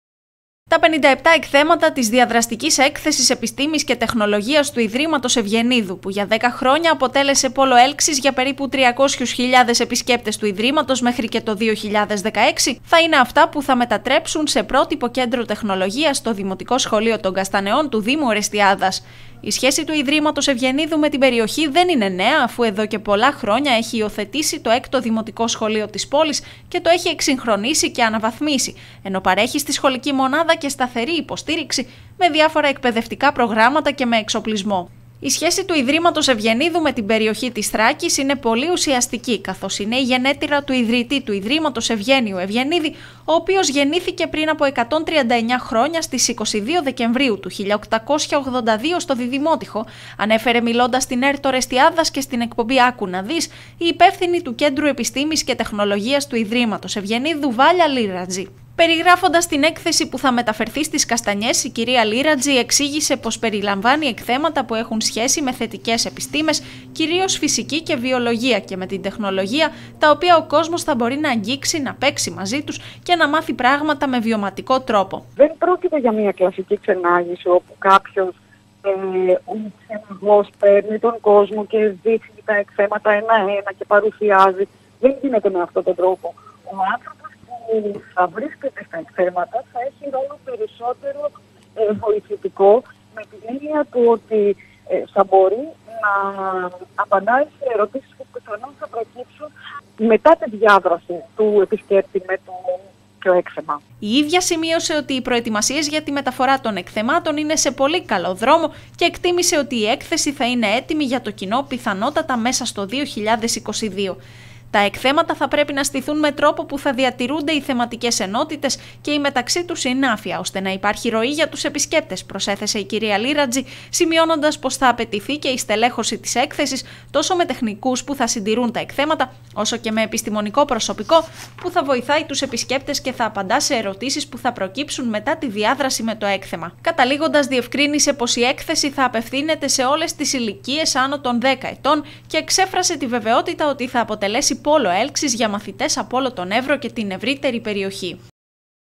Η διαδραστική έκθεση Επιστήμης Τεχνολογίας του ιδρύματος Ευγενίδου στο σχολείο Καστανεών
Εντός του 2022 αναμένεται να ανοίξει τις πόρτες της η Έκθεση Επιστήμης και Τεχνολογίας του Ιδρύματος Ευγενίδου, στο σχολείο των Καστανεών στον Έβρο.
Τα 57 εκθέματα της διαδραστικής έκθεσης που για 10 χρόνια αποτέλεσε πόλο έλξης για περίπου 300.000 επισκέπτες του Ιδρύματος, μέχρι και το 2016, θα είναι αυτά που θα μετατρέψουν σε Πρότυπο Κέντρο Τεχνολογίας το δημοτικό σχολείο των Καστανεών του δήμου Ορεστιάδας.